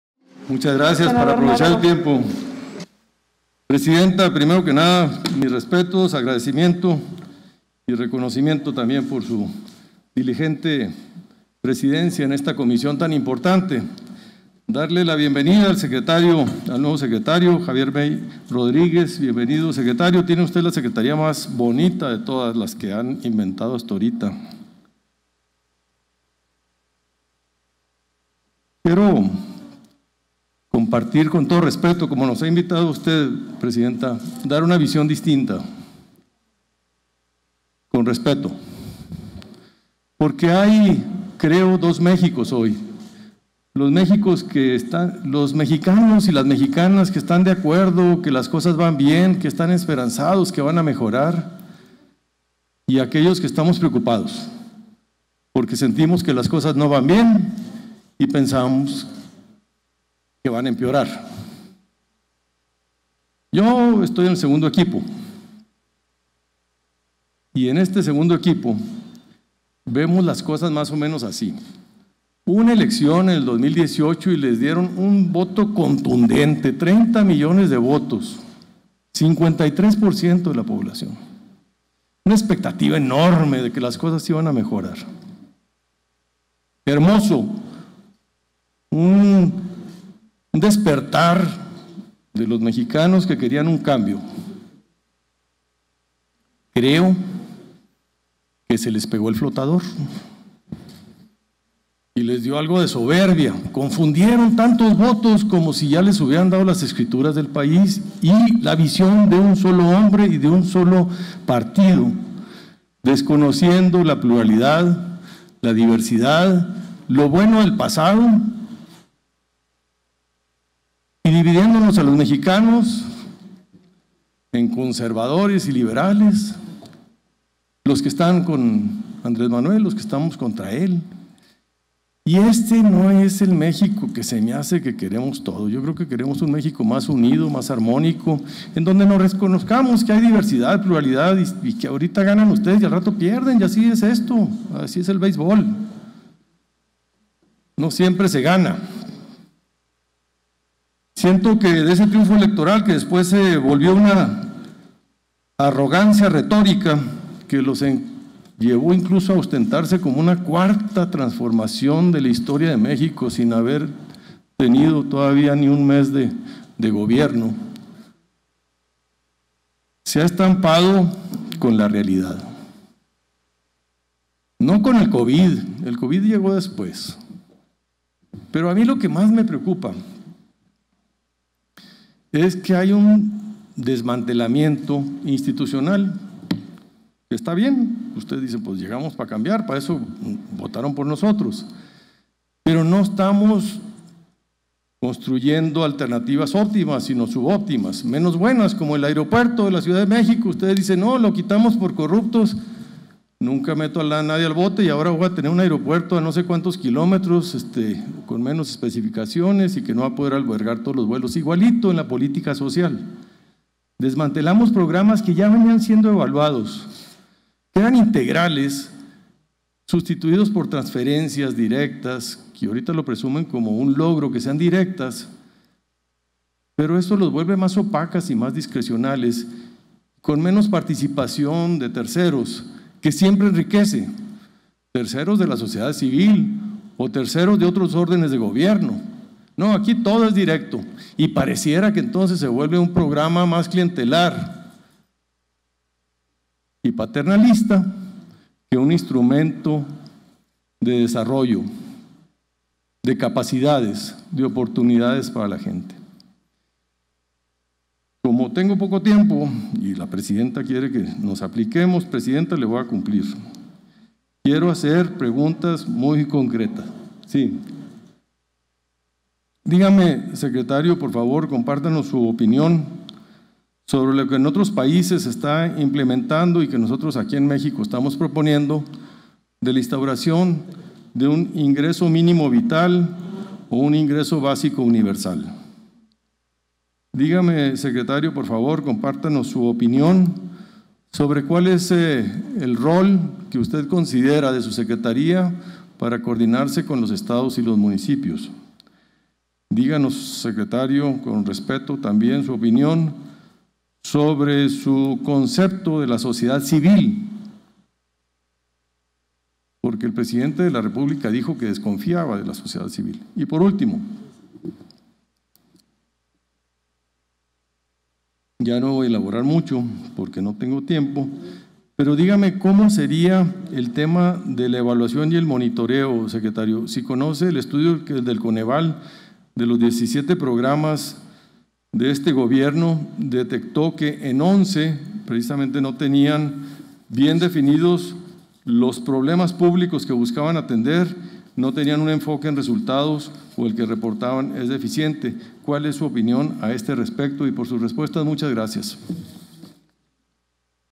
Senador Gustavo Madero Muñoz, durante la comparecencia del secretario del Bienestar: Segundo Informe
29 de septiembre de 2020.- Intervención del senador Gustavo Madero Muñoz, durante la comparecencia del secretario del Bienestar, Javier May Rodríguez, con motivo del análisis del Segundo Informe de Gobierno.